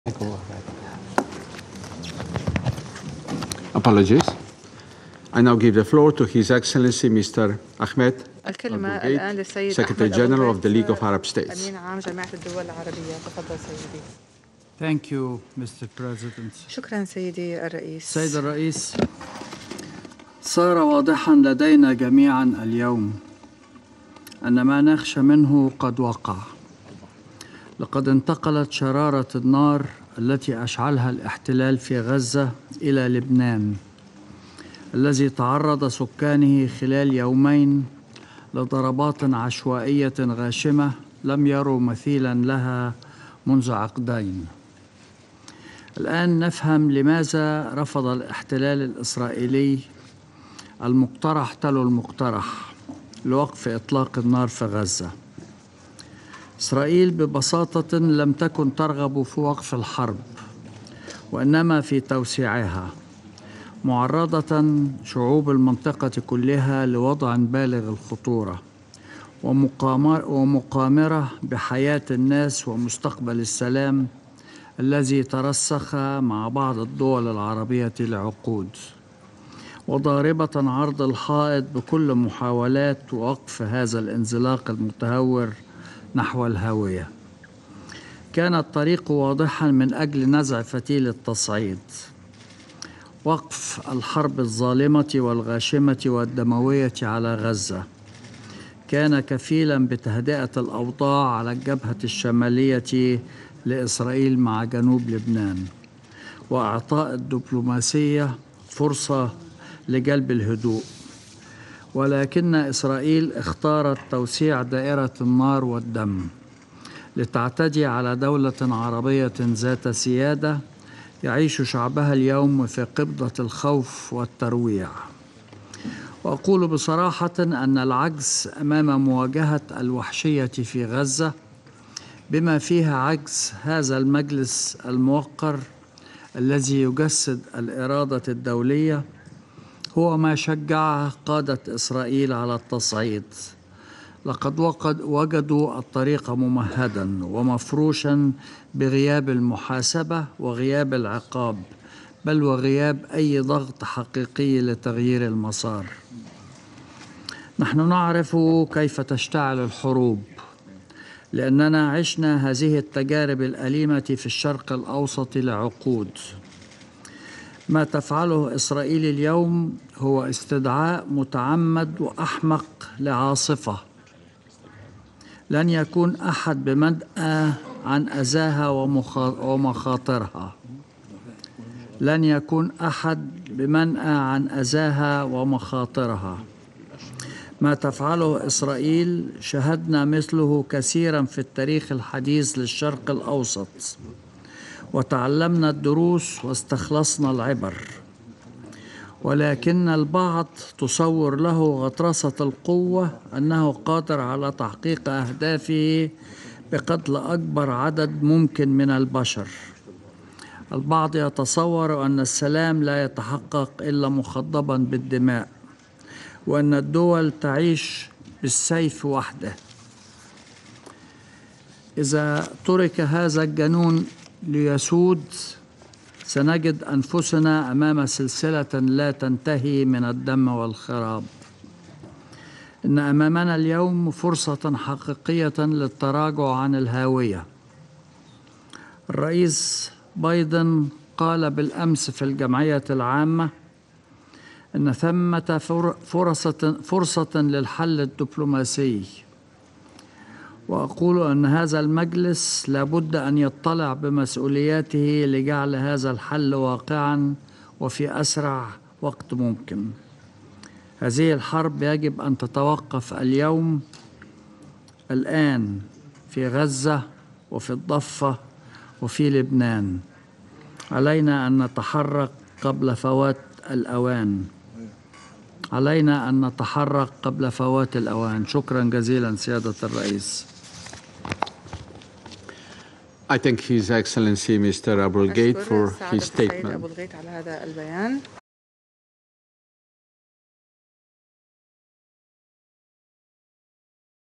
أبو الغيط في مجلس الأمن: علينا التحرك قبل فوات الأوان
"الحرب يجب أن تتوقف الآن في غزة والضفة الغربية ولبنان.. وعلينا التحرك قبل فوات الأوان".. كلمة أمين عام الجامعة العربية أحمد أبو الغيط أمام مجلس الأمن